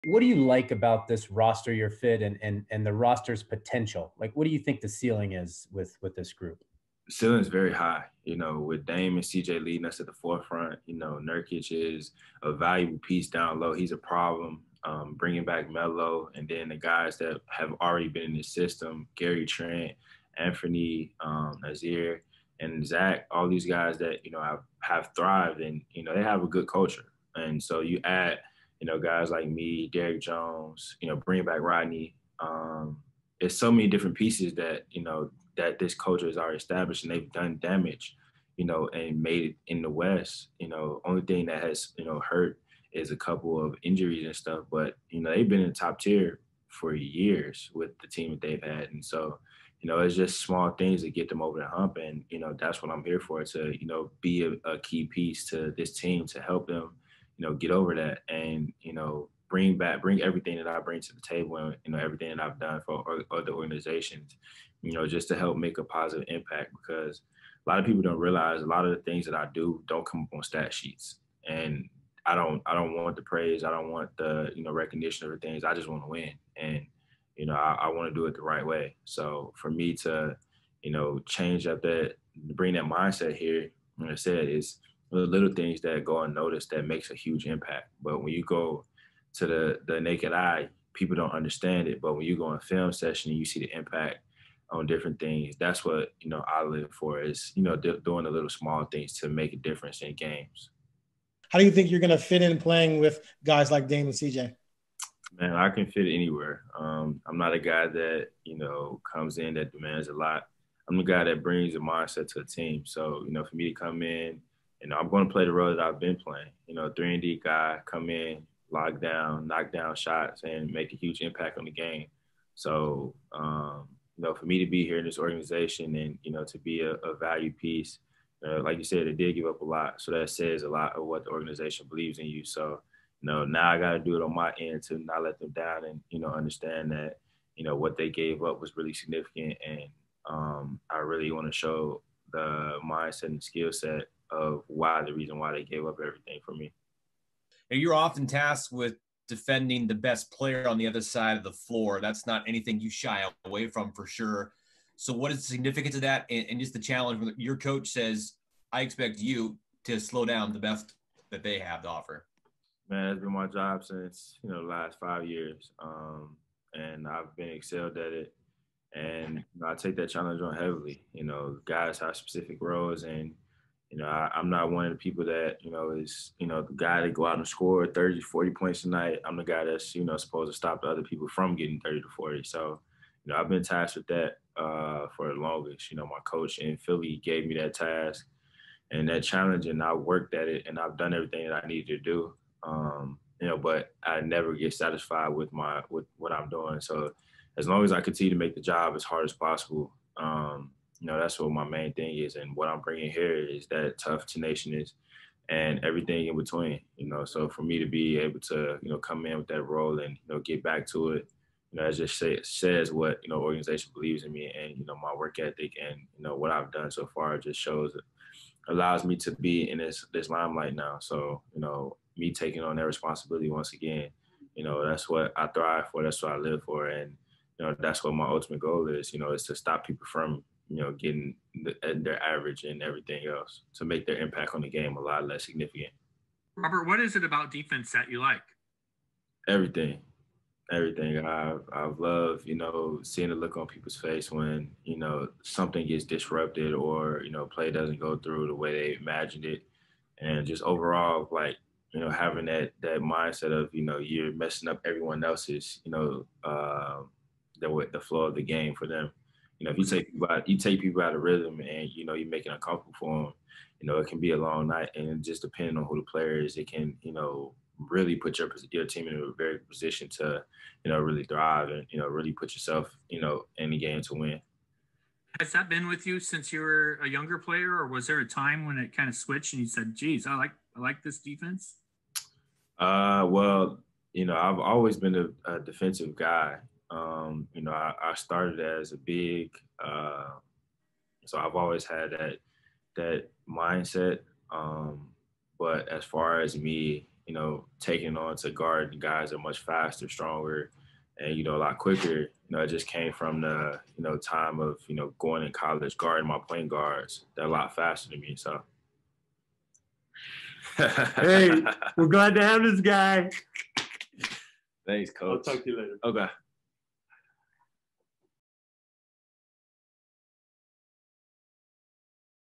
Portland Trail Blazers forward Robert Covington embraces defensive identity
Robert Covington, acquired from the Houston Rockets last week, discusses his fit with the Portland Trail Blazers and embracing his defensive identity.